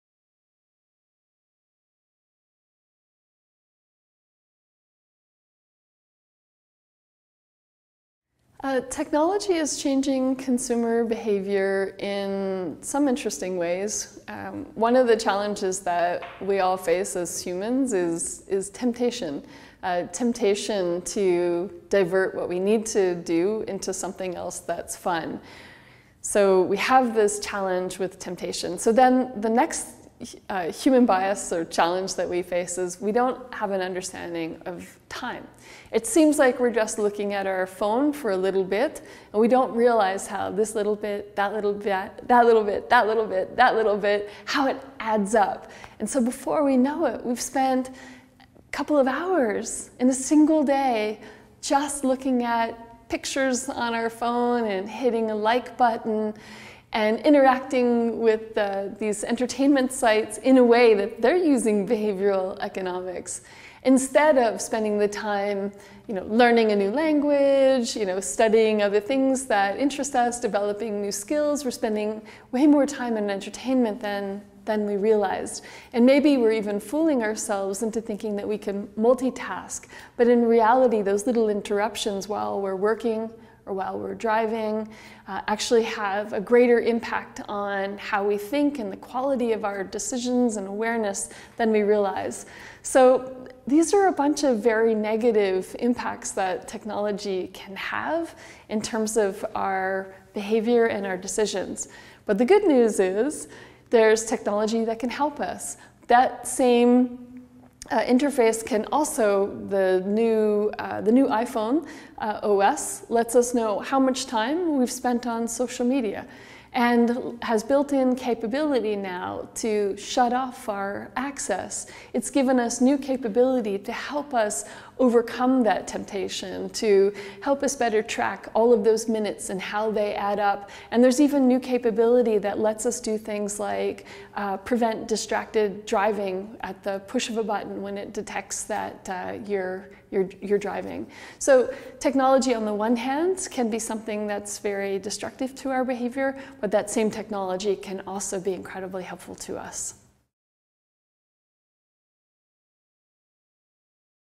Kelly Peters - Technology and consumer behavior
For Kelly Peters, CEO and founder of BEworks, technology is changing consumer behavior in many ways. Learn more by watching this video.